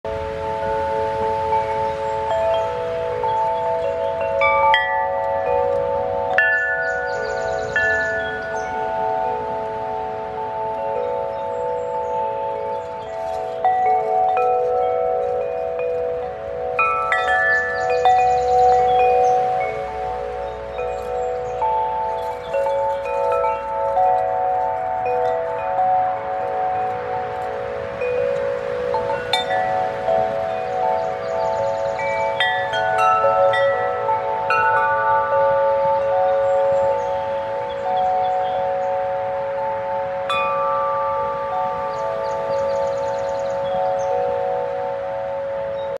A minute of Peace from The Bishop Baraga Shrine 6/30/2020
Early this morning as I looked out over the sparkling blue waters of Keweenaw Bay-the chimes started moving in the gentle breeze-soon the beautiful sound resonated -surrounding me with peace and a promise of a wonderful day in Baraga-one of God's "special places"!  We are truly blessed to live here!